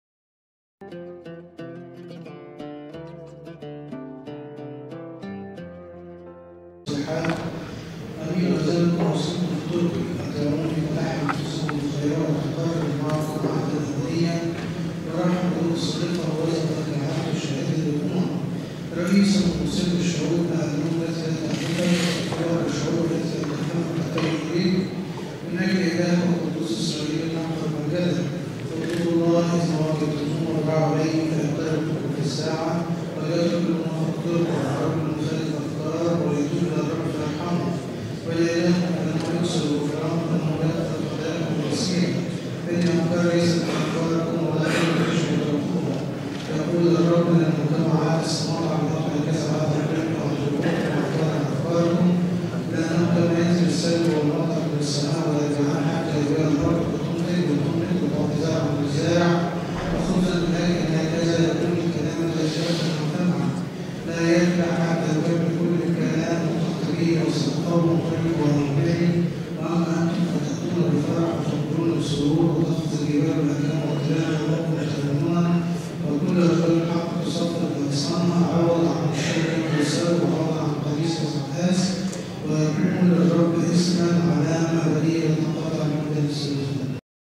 الآباء الرهبان بدير السريان يصلون نبؤة عن أشعياء النبى فى باكر سبت الفرح فى ليلة أبوغالمسيس
من أشعياء النبي، بركته المقدسة تكون معنا. آمين.
Isaiah 55:2-13
ص ٥٥: ٢ -١٣
Hearken to me, and ye shall eat that which is good, and your soul shall feast itself on good things.
اسمعوا لي فتأكلوا الخيرات ولتتلذذ نفوسكم بالصالحات.
Give heed with your ears, and follow my ways: hearken to me, and your soul shall live in prosperity; and I will make with you an everlasting covenant, the sure mercies of David. Behold I have made him a testimony among the Gentiles, a prince and commander to the Gentiles. Nations which know thee not, shall call upon thee, and peoples which are not acquainted with thee, shall flee to thee for refuge, for the sake of the Lord thy God, the Holy One of Israel; for he has glorified thee.
أميلوا آذانكم واسلكوا في طرقي أطيعوني فتحيا نفوسكم بالخيرات أقرر معكم عهدا أبديا وأمنحكم مكرمات داود الصادقة هوذا قد جعلته شاهداً للأمم ورئيسا وموصيا للشعوب. ها الأمم التي لا تعرفك تطلبك والشعوب التي لم تفهمك تلتجئ إليك من أجل أن الهك قدوس اسرائيل لأنه قد مجدك.

Seek ye the Lord, and when ye find him, call upon him; and when he shall draw nigh to you, let the ungodly leave his ways, and the transgressor his counsels: and let him return to the Lord, and he shall find mercy; for he shall abundantly pardon your sins. For my counsels are not as your counsels, nor are my ways as your ways, saith the Lord. But as the heaven is distant from the earth, so is my way distant from your ways, and your thoughts from my mind. For as rain shall come down, or snow, from heaven, and shall not return until it have saturated the earth, and it bring forth, and bud, and give seed to the sower, and bread for food: so shall my word be, whatever shall proceed out of my mouth, it shall by no means turn back, until all the things which I willed shall have been accomplished; and I will make thy ways prosperous, and will effect my commands. For ye shall go forth with joy, and shall be taught with gladness: for the mountains and the hills shall exult to welcome you with joy, and all the trees of the field shall applaud with their branches. And instead of the bramble shall come up the cypress, and instead of the nettle shall come up the myrtle: and the Lord shall be for a name, and for an everlasting sign, and shall not fail.
اطلبوا الله وإذا ما وجدتموه اضرعوا إليه فيقترب منكم في الساعة وليترك المنافق طرقه والرجل المخالف أفكاره وليأت إلى الرب فيرحمه وإلى إلهنا فإنه يكثر الغفران فإنه يغفر خطاياكم الكثيرة فإن أفكارى ليست أفكاركم ولا طرقي تشبه طرقكم يقول الرب، لأنه كما علت السموات عن الأرض هكذا علت طرقي عن طرقكم وأفكارى عن أفكاركم لأنه كما ينزل الندة والمطر من السماء ولا يرجعان حتى يرويان الأرض فتنتج وتنبت وتعطي زرعاً للزارع وخبزا للآكل هكذا يكون الكلام الذي يخرج من فمي لا يرجع حتى يكمل كل كلام نطقت به، وسأقوم طرقي وأوامرى وأما أنتم فتأتون بفرح وتقبلون بسرور وتقفز الجبال والآكام والتلال أمامكم ترنما وكل اشجار الحقل تصفق بأغضانها عوضا عن الشوك ينبت سرو. وعوضا عن القريس يصعد آس ويكون للرب اسما علامة أبدية لا تنقطع.
Glory be the Holy Trinity, our God, forever and ever. Amen.
مجداً للثالوث المقدس إلهنا إلى الأبد وإلى أبد الآبدين كلها. آمين.